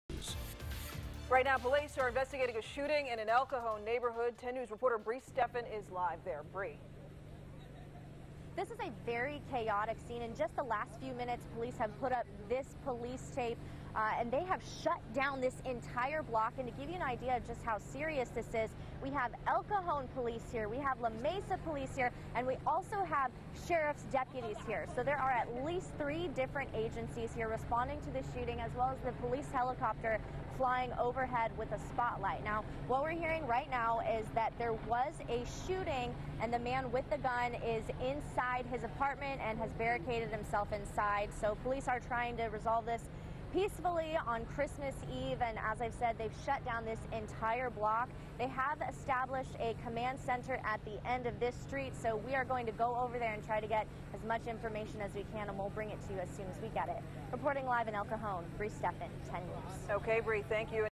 Man fires shots, holds woman hostage in El Cajon
A man holding a woman hostage fired shots then barricaded himself into an apartment for a three-hour standoff Thursday night before surrendering himself to police.